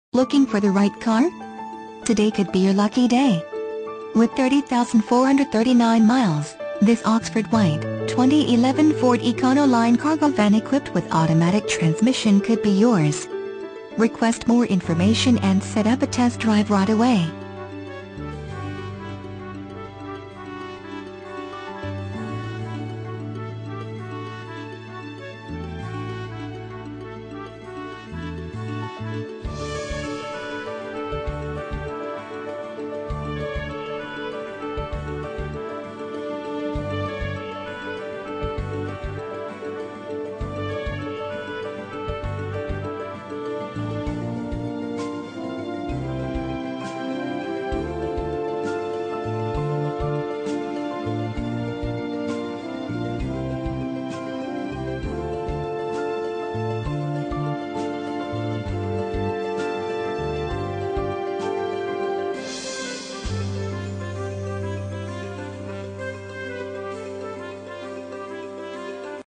2011 FORD ECONOLINE CARGO VAN Cleveland, GA P6709
2011 FORD ECONOLINE CARGO VAN Cleveland, GA
Stock

Looking for the right car? Today could be your lucky day. With 30439 miles, this Oxford White, 2011 Ford Econoline Cargo Van equipped with automatic transmission could be yours. Request more information and set up a test drive right away.